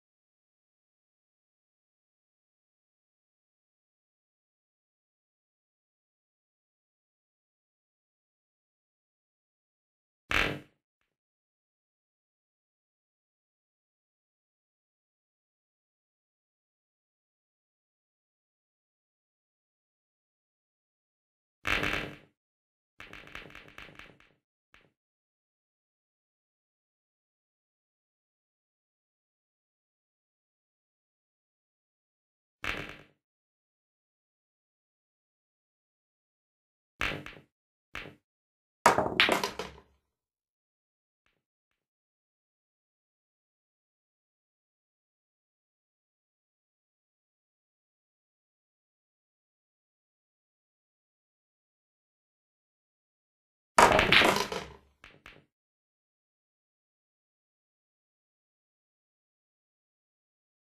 Arcade pool - XNA Game
A quick 3D Xna pool game I made during the January 2009 holidays for my first year at the University of Derby.